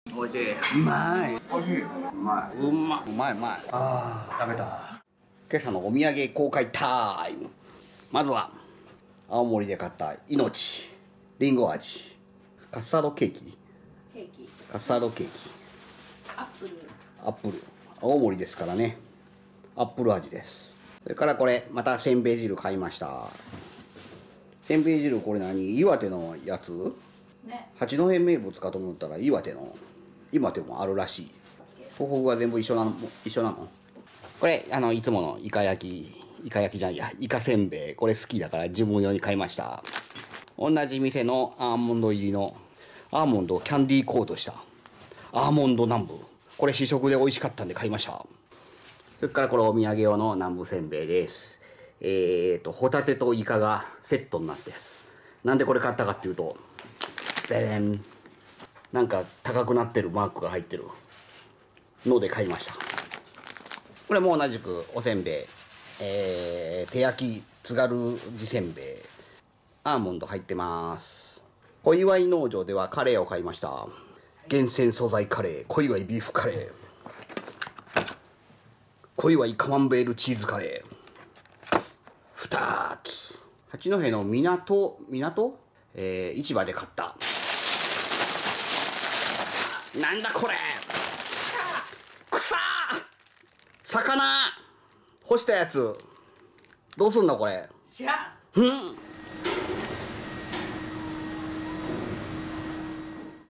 北三陸 青森・岩手の旅 岩手のおみやげ,Tohoku Journey:Iwate souvenir
北三陸 青森・岩手 満腹中枢破壊の旅 #30 岩手のおみやげ盛岡駅編

みみさん北三陸 青森・岩手 北東北 満腹中枢破壊の旅行記：目次

岩手のおみやげ

青森、岩手でもお買い物をいっぱいしました。
お土産や自分用など。

お菓子や、食材や、おつまみ。
東北はおいしいものがいっぱいですね。

ついついいろいろ買っちゃいます。

I did some shopping in Aomori and Iwate.
Souvenir.

The Tohoku district is full of delicious food.
I bought many goods.

60pでの撮影です。
編集はEDIUS 6.5です。

MIMINOME-MOVIE
みみさん
Mimi-san